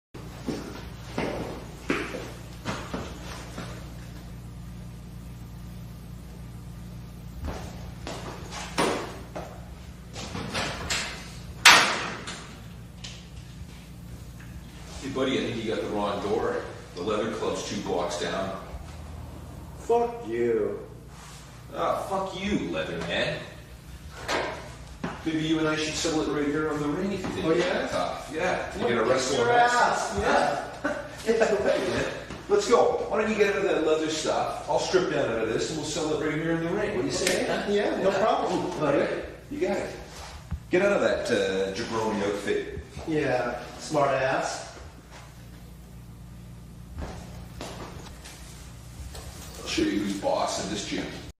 datamosh experiment: ♂Inugami Korone intro♂
Experimeting with datamoshing viso streams...
Removed all I-frames to break scene (pov) changes.
Nearby P-frames swapped with 10% probability to initiate more broken macroblocks at moving parts.
These operations done two times, with lossles re-encoding between passes to restore I-frames.
This actually looks exatly how I imagine perfect datamosh.